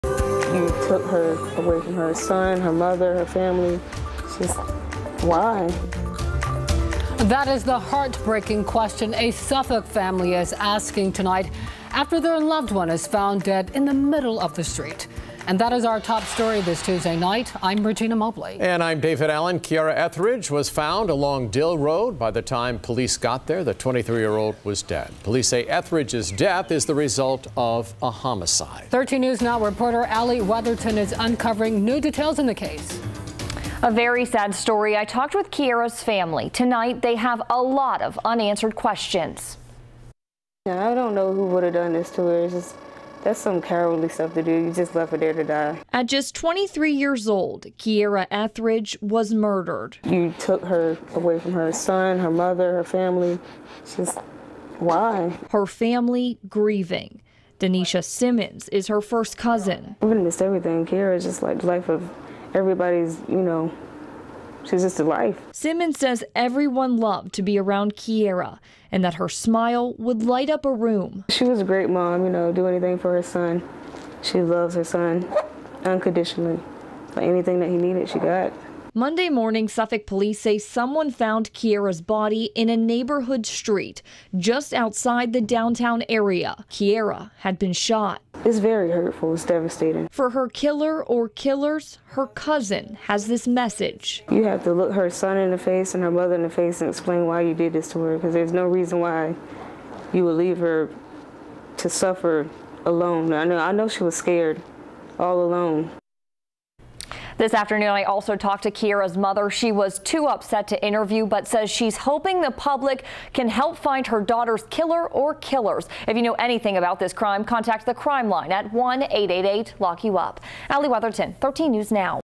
Family wants answers in 23-year-old's killing
A Suffolk family is asking for answers after a 23-year-old was found dead in the middle of the street.